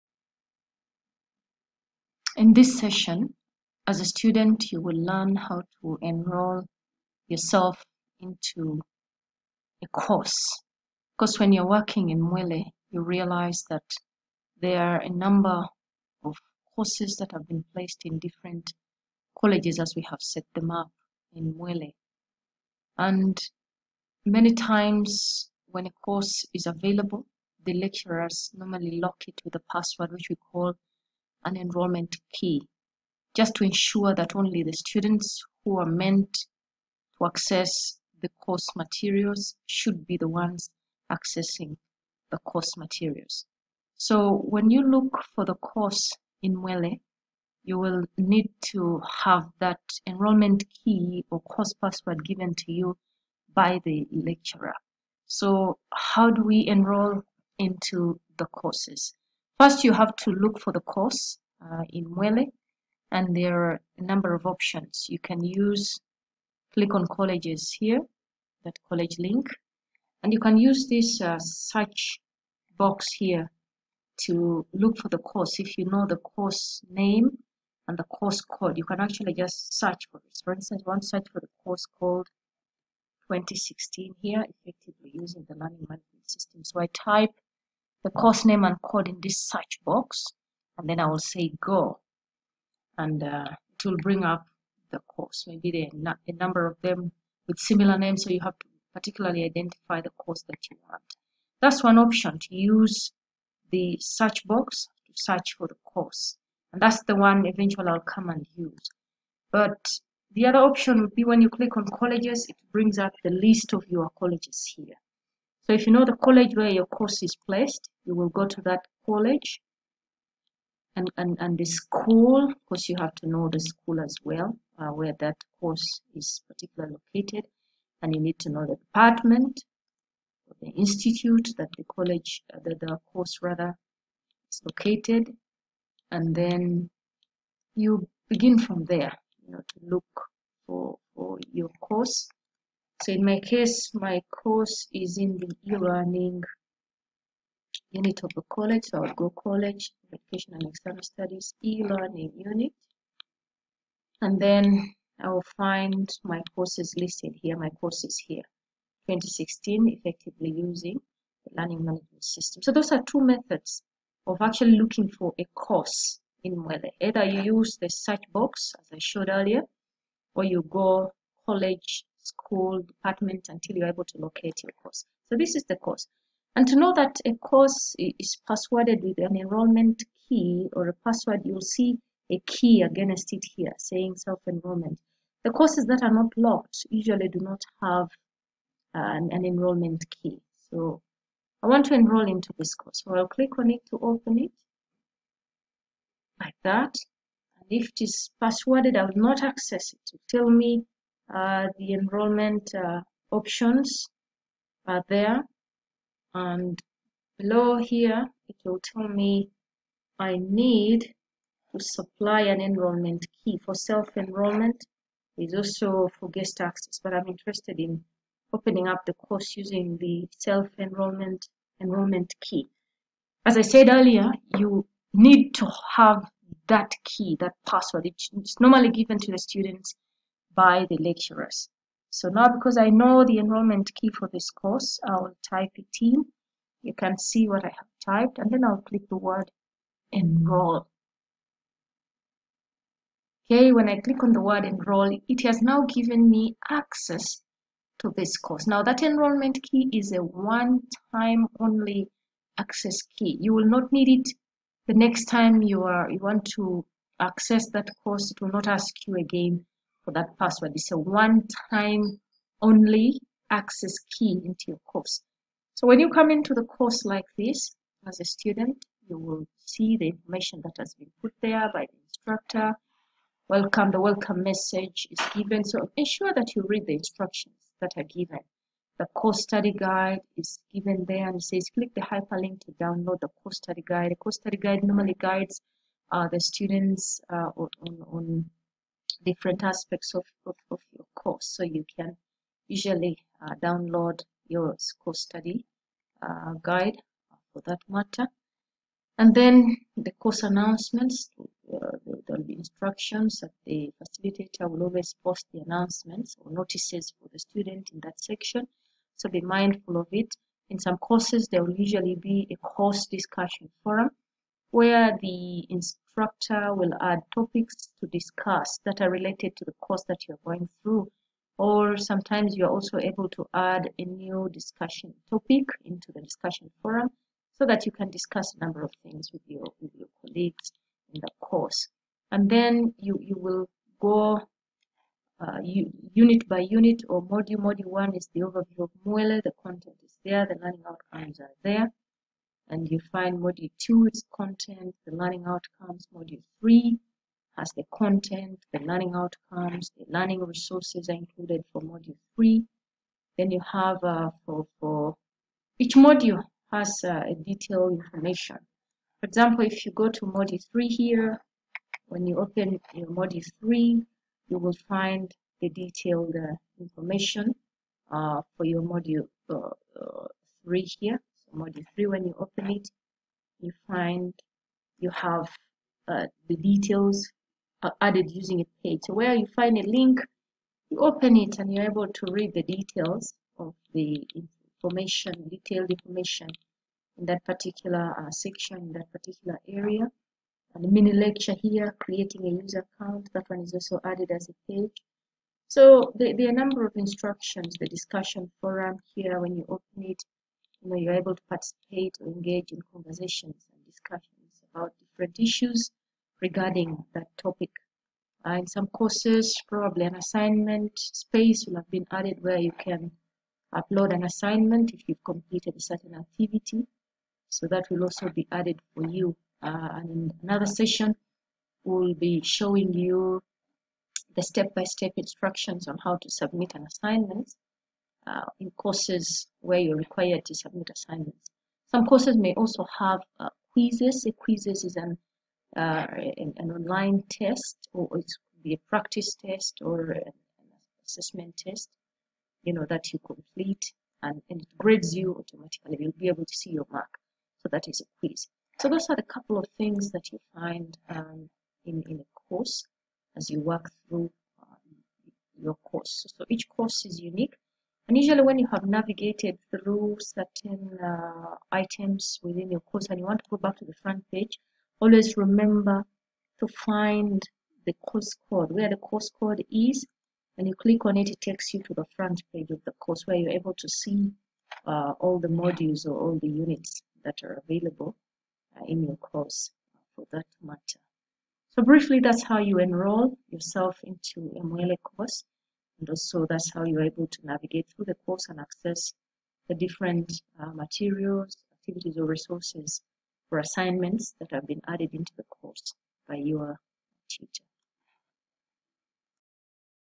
MUELE (Students) How to enroll into a Course
Instructions on how to enroll into a  MUELE course using an enrollment key and how to navigate through the course